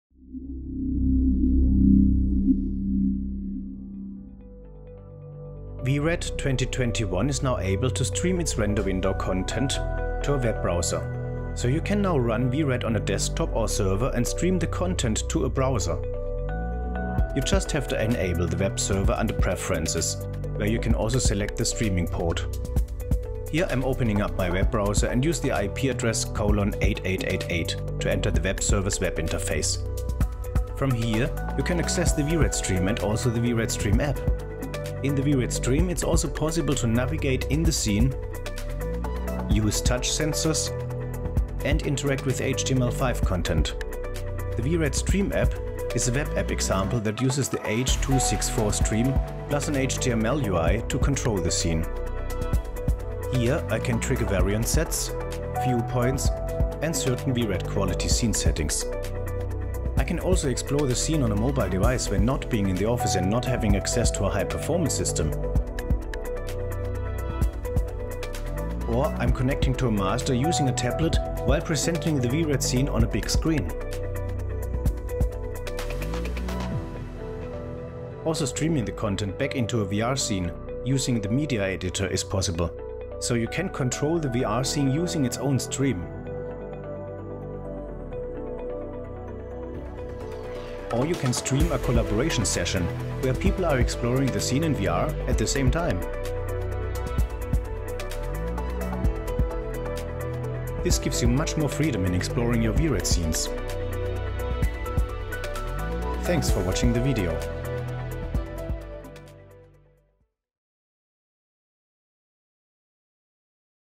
Vred 2021: what's new (4/5) ?
Découvrez Vred: le logiciel de visualisation 3D VRED™ qui permet aux concepteurs et aux ingénieurs de créer des présentations de produits, des révisions de conceptions et des prototypes virtuels à l'aide du lancer de rayons interactif via processeur graphique, ainsi que des modes de rendu analytique et cloud.